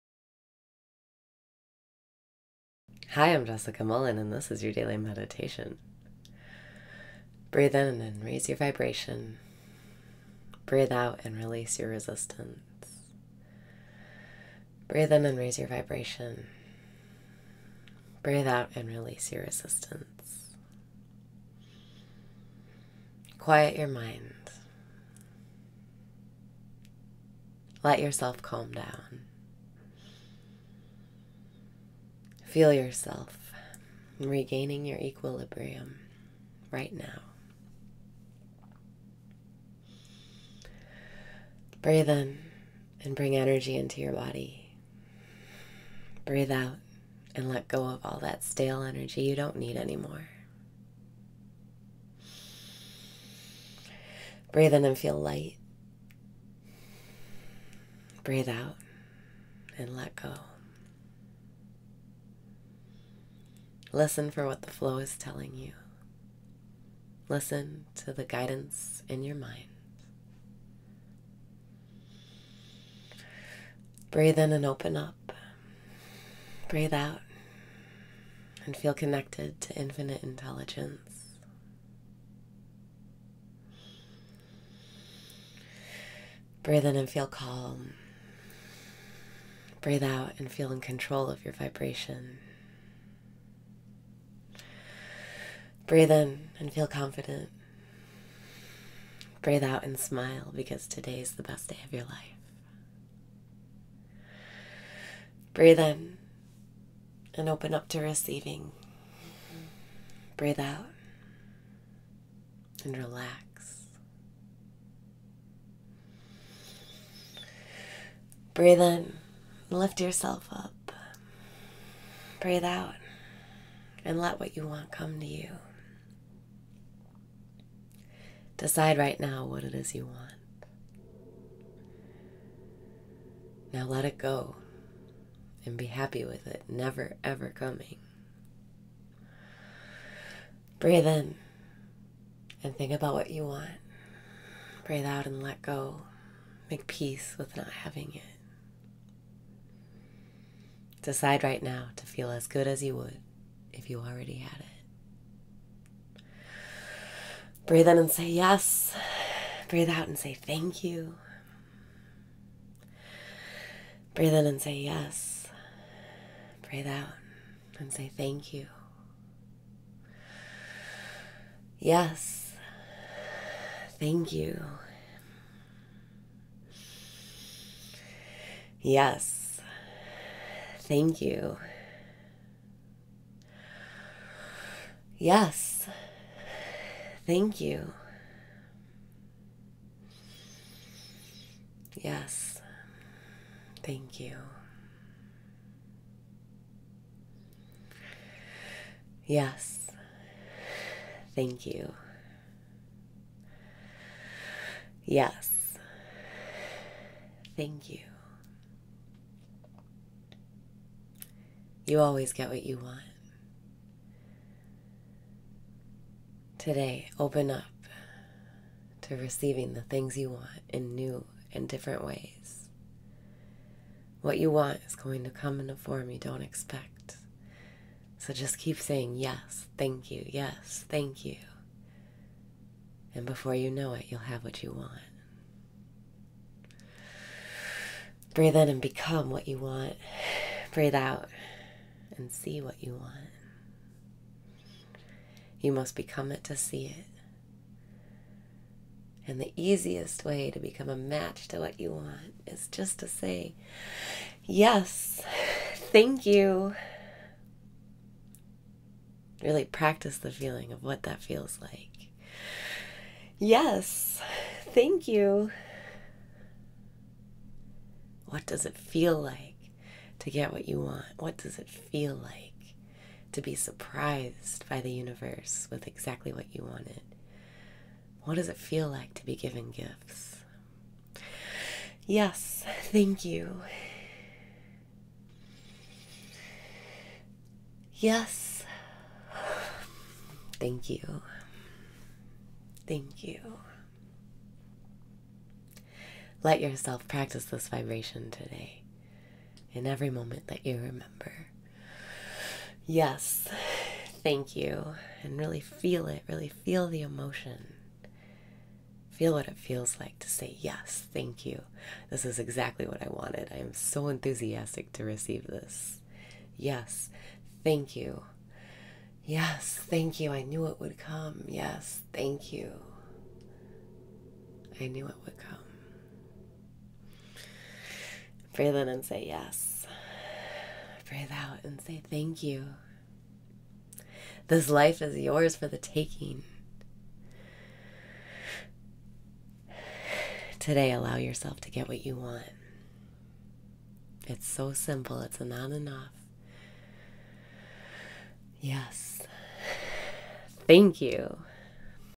Daily Meditation - What Does It Feel Like to Get What You Want?
Quiet your mind. Let yourself calm down. Feel yourself regaining your equilibrium right now.

Breathe in and bring energy into your body
Breathe out and let go of all that stale energy you don’t need anymore

Breathe in and feel light

Listen for what the flow is telling you. Listen to the guidance in your mind.

Breathe in and open up
Breathe out and feel connected to infinite intelligence

Breathe in and feel calm
Breathe out and feel in control of your vibration

Breathe in and feel confident
Breathe out and smile because today is the best day of your life

Breathe out and let what you want come to you

Decide right now what it is you want. Now let it go and be happy with is never ever coming.

Breathe out and let go. Make peace with not having it

Decide right now to feel as good as you would if you already had it

YES!

YES!

You always get what you want. Today, open up to receiving the things you want in new and different ways. What you want is going to come in a form you don’t expect so just keep saying YES THANK YOU YES THANK YOU! And before you know it you’ll have what you want.

Breathe in and become what you want!
Breathe out and see what you want

You must become it to see it. The easiest way to become a match to what you want is just to say YES THANK YOU! Really practice the feeling of what that feels like. YES THANK YOU! What does it feel like to get what you want? What does it feel like to be surprised by the universe with exactly what you wanted? What does it feel like to be given gifts? YES! THANK YOU! YES! Thank you!

Let yourself practice this vibration today. In every moment that you remember: YES! THANK YOU! Really feel the emotion. Feel what it feels like to say YES THAK YOU! This is exactly what I wanted. I am so enthusiastic to receive this. YES THANK YOU! I knew it would come. YES THANK YOU! I knew it would come.

This life is yours for the taking. Today, allow yourself to get what you want. It’s so simple—it’s an on and off. YES! THANK YOU!